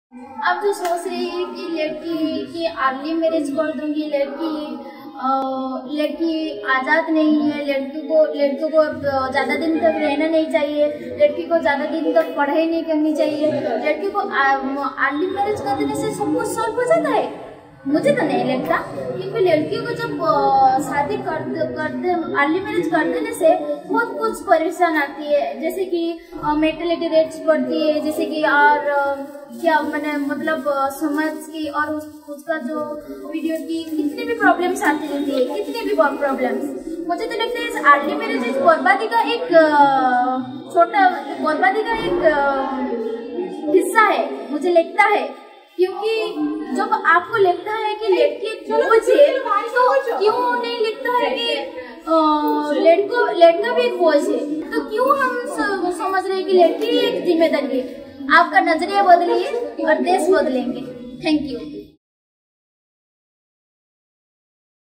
MarziBinaShaadiNahi | Sasmita Mandal, Odisha
Early and Forced marriages have plagued India for decades.

Oxfam India is campaigning to raise awareness about the issues related to Early and Forced Marriages. Our supporters in our focus states are raising their voices against Early and Forced Marriages.